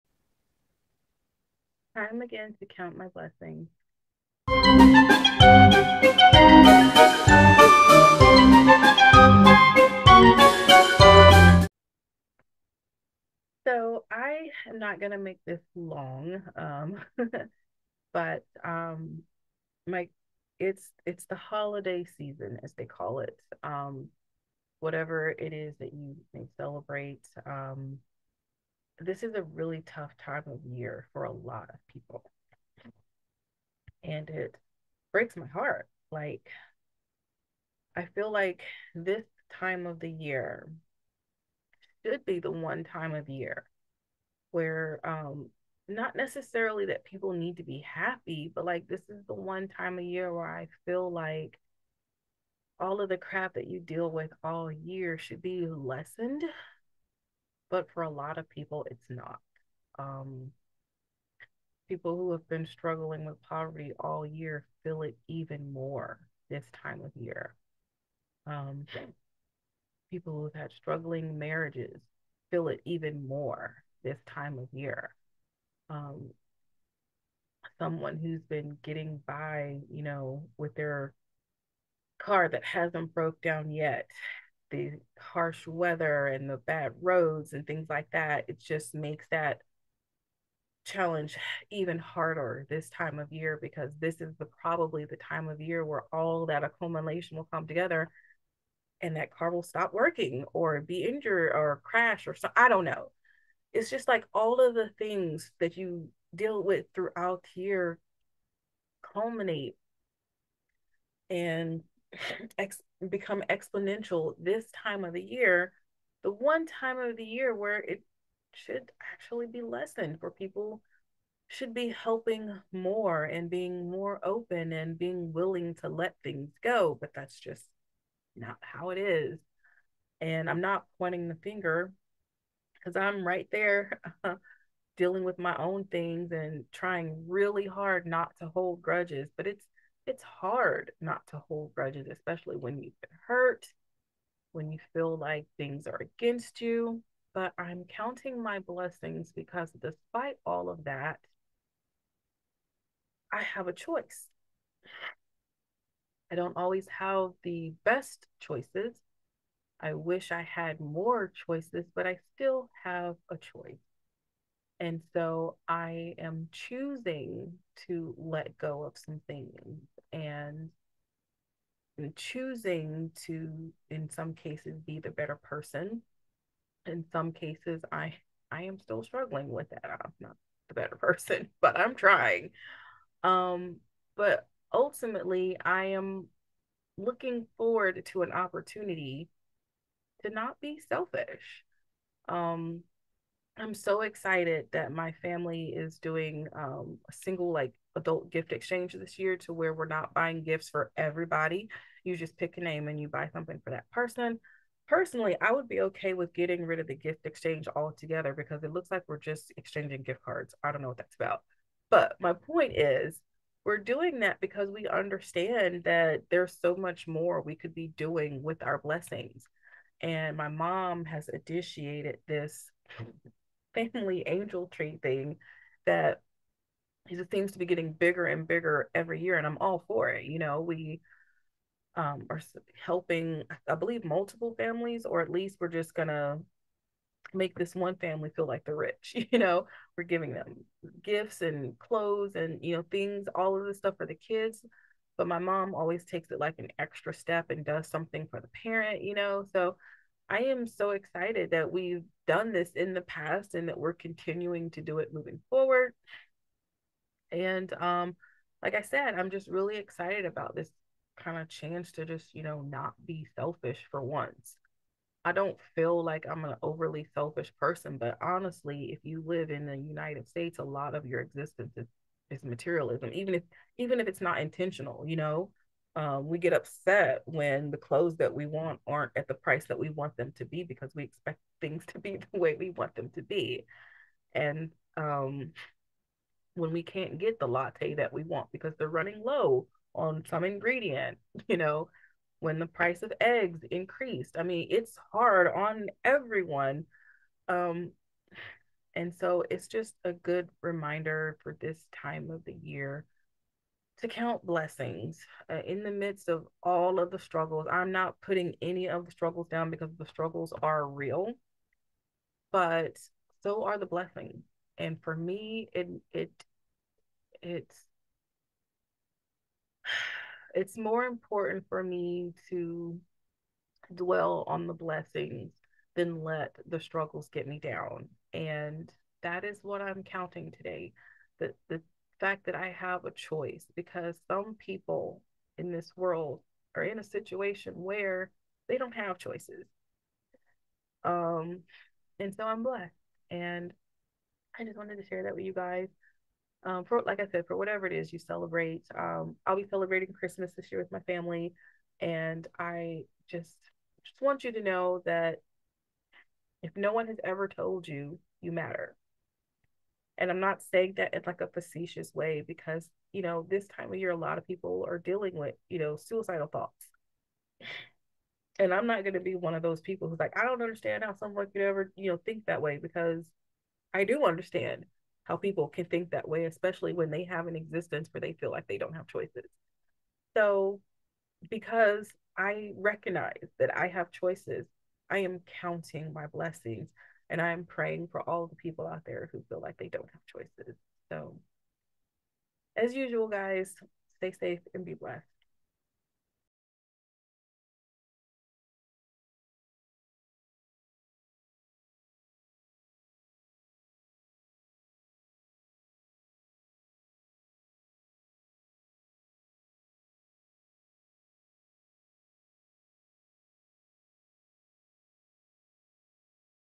Counting My Blessings
This should be the season of love and cheer but some people are struggling right now. Today I count my blessings and send out a prayer to those in need.
Follow for FREE and receive a monthly video update.